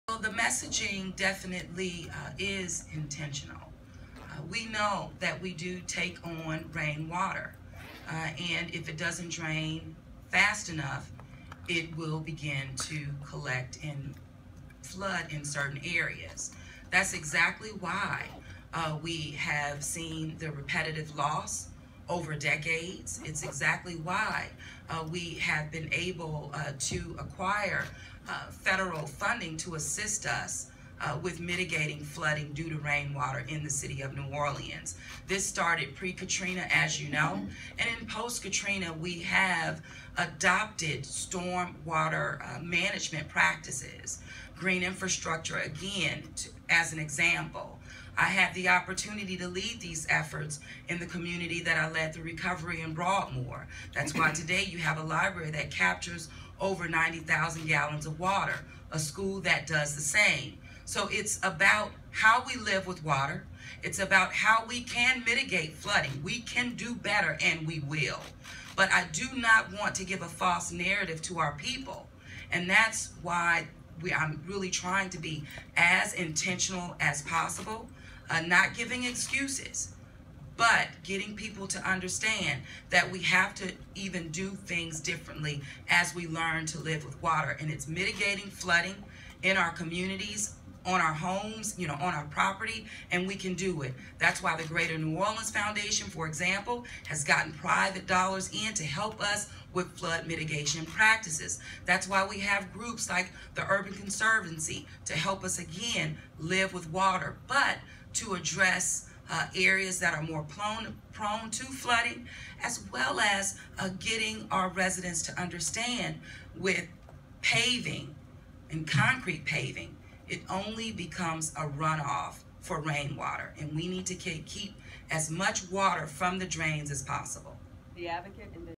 Mayor Cantrell addresses how New Orleans must co-exist with water
Early in her New Orleans administration, Mayor LaToya Cantrell is taking a realistic approach to the city's history with flooding. ”It's about how we live with water. It's about how we can mitigate flooding. We can do better and we will."